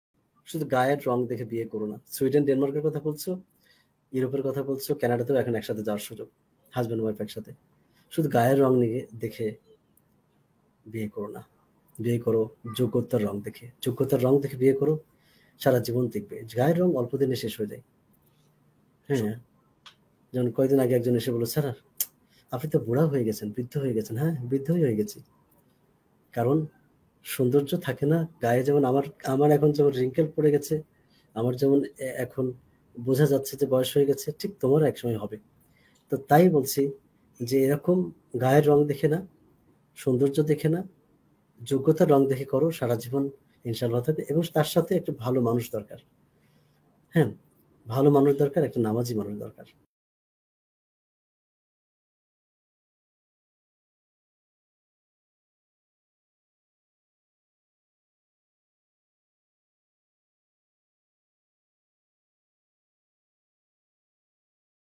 বিয়ের ক্ষেত্রে গায়ের রং নাকি যোগ্যতার রং ! || ATM sir || ATM Mahmud
এই ২ মিনিট ৫১ সেকেন্ডেই আপনার জীবন পরিবর্তন করতে পারে ইনশাআল্লাহ। মনোযোগ দিয়ে প্রতিটা কথা শুনুন এবং সে অনুযায়ী জীবন পথের কাজে লাগবে। দেখবেন আপনিও সফল হবেন ইনশাআল্লাহ।